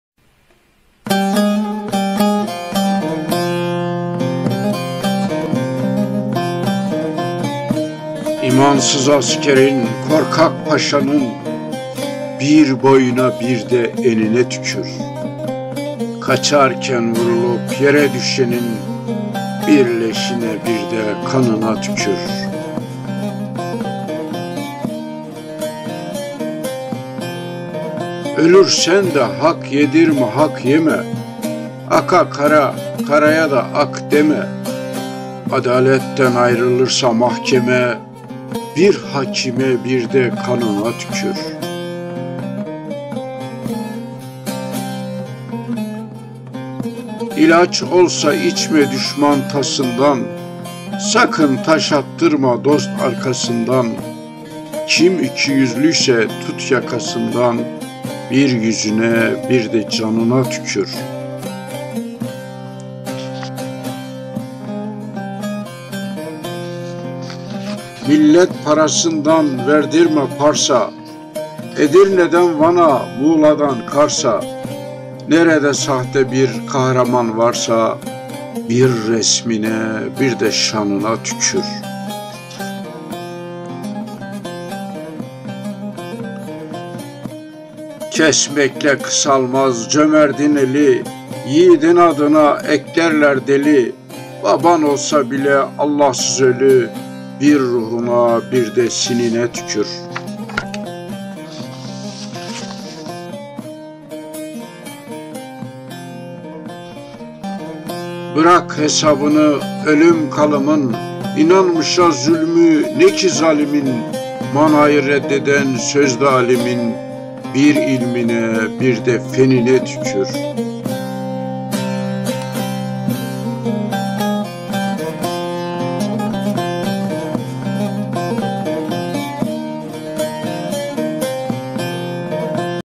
Vasiyet - Abdurrahim Karakoç - Seçme Şiirler
Değerli Şairimiz Abdurrahim Karakoç'un yazdığı "Vasiyet" adlı şiir İsa Akbulak tarafından seslendirilmiştir.